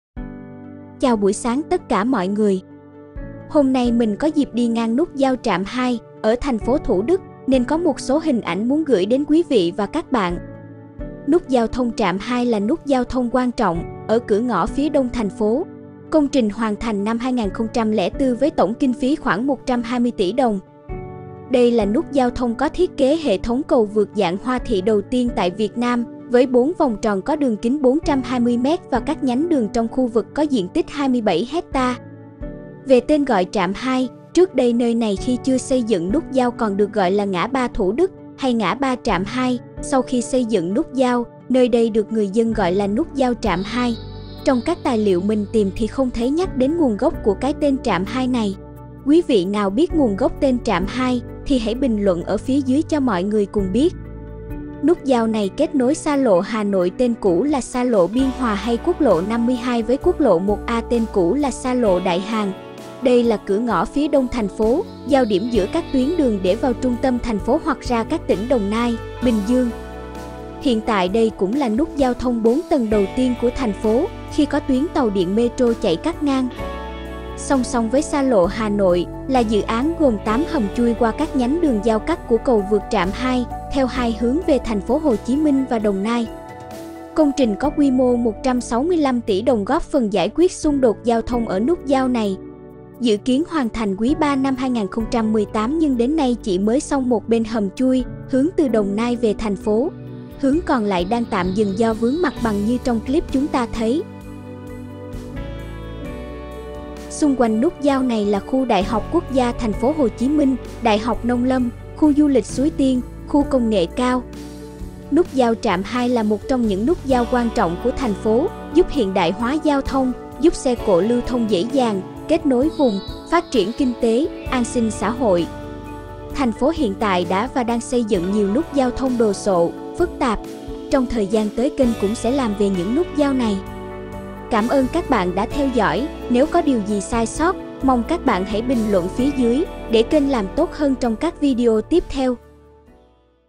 Nút giao trạm 2 | Nút giao 4 tầng đầu tiên của Thành phố Hồ Chí Minh | Saigon71
Nút giao thông trạm 2 là nút giao thông quan trọng ở của ngõ phía Đông thành phố. Công trình hoàn thành năm 2004 với tổng kinh phí khoảng 120 tỷ đồng.
Đây là nút giao thông có thiết kế hệ thống cầu vượt dạng hoa thị đầu tiên tại Việt Nam, với bốn vòng tròn có đường kính 420 mét và các nhánh đường trong khu vực có diện tích 27 ha.

Nút giao này kết nối Xa lộ Hà Nội (tên cũ là xa lộ Biên hòa hay quốc lộ 52) với quốc lộ 1A (tên cũ là xa lộ Đại Hàn). Đây là cửa ngõ phía đông thành phố, giao điểm giữa các tuyến đường để vào trung tâm thành phố hoặc ra các tỉnh Đồng Nai, Bình Dương...

Hiện tại đây cũng là nút giao thông 4 tầng đầu tiên của Thành phố, khi có tuyến tàu điện metro chạy cắt ngang.

Song song với Xa lộ Hà Nội, là dự án gồm 8 hầm chui qua các nhánh đường giao cắt của cầu vượt Trạm 2, theo hai hướng về thành phố hồ chí mình và Đồng Nai.
Công trình có quy mô 165 tỷ đồng góp phần giải quyết xung đột giao thông ở nút giao này; khởi công tháng 5/2017, dự kiến hoàn thành quý 3/2018 nhưng đến nay chỉ mới xong 1 bên hầm chui, hướng từ đồng nai về thành phố.
Bản quyền thuộc về Saigon71
© Saigon71